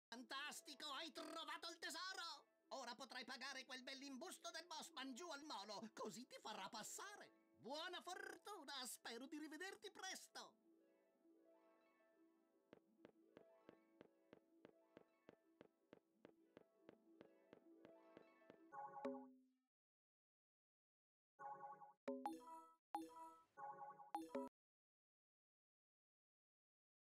pirate skip
another skip i probably didn't need to make a video for. after the cutscene select "replay level" and then open the menu and select "return to hub"